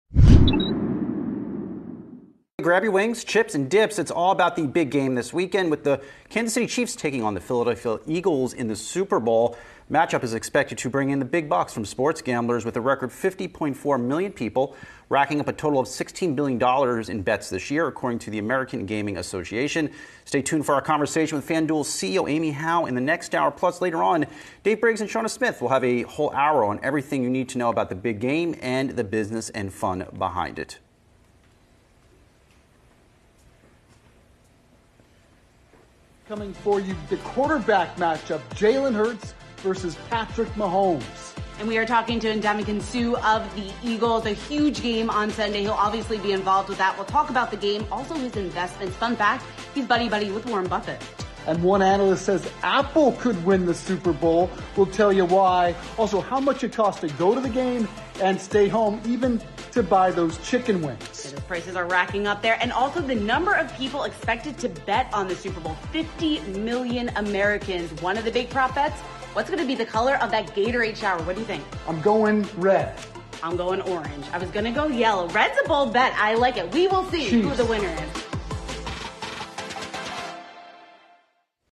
Super Bowl 2023: 50.4 million bettors wager on the big game
Yahoo Finance Live anchors break down Super Bowl sports betting and what to know ahead of the Eagles-Chiefs matchup. Don't miss the Yahoo Finance Super Bowl special February 10, 2023 at 4pm EST.